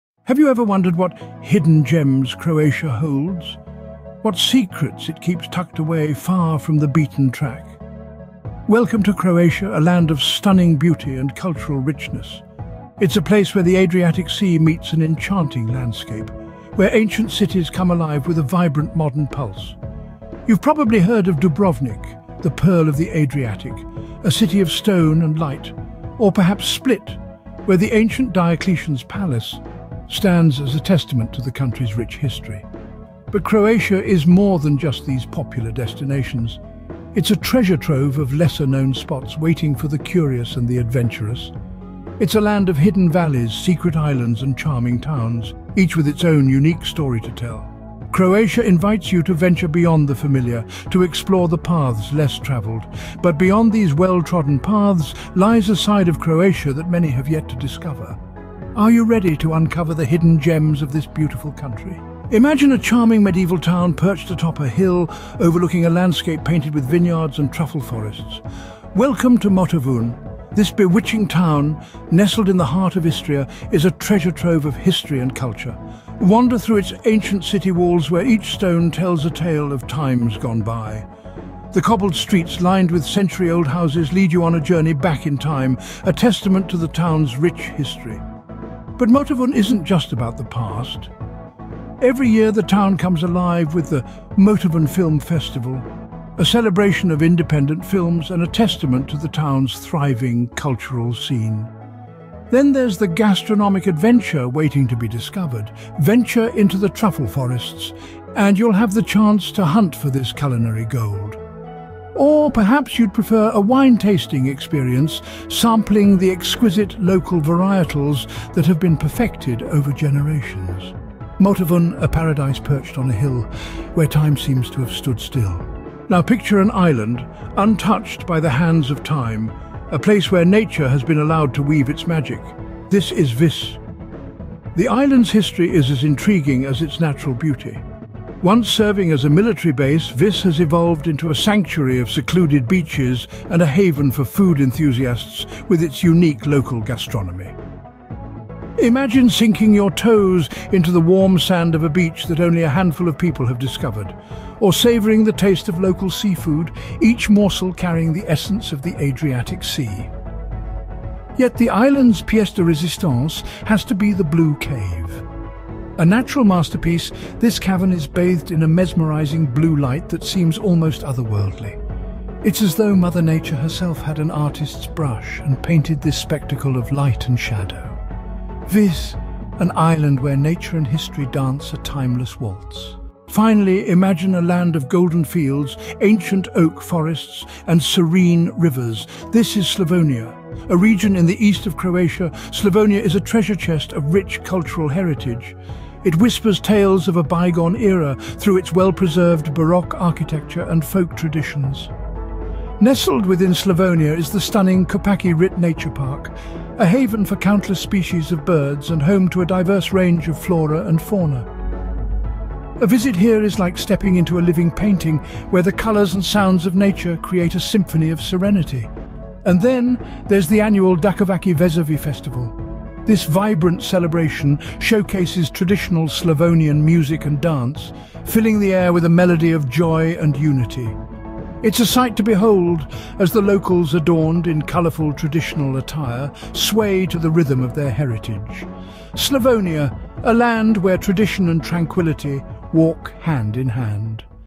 Hidden Gems of Croatia
In this video, prepare to be amazed as we explore the hidden gems of Croatia that are sure to take your breath away. From secluded beaches to charming villages, we uncover the lesser-known yet stunning locations that Croatia has to offer.

Join us on this exciting journey as we venture off the beaten path and showcase the beauty of Croatia beyond the popular tourist spots. Get ready to be inspired by the picturesque landscapes and rich cultural heritage that make Croatia a truly unique destination.

OUTLINE:

00:00:00 Unfolding the Hidden Gems of Croatia
00:01:02 Discovering Motovun
00:02:05 The Mystical Island of Vis
00:03:03 The Tranquility of Slavonia